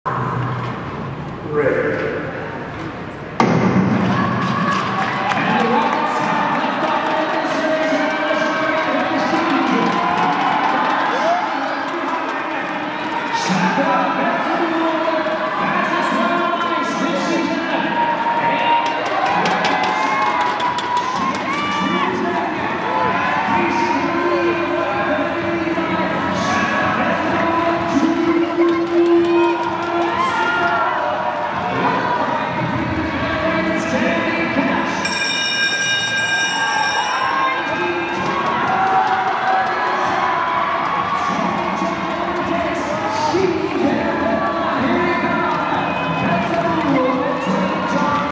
[최민정] 2022/23 ISU 쇼트트랙 월드컵 2차 여자 500m (2) 준결승 (직캠)
ISU World Cup 2022/23 - Salt Lake City (USA)
Women 500m (2) Semifinals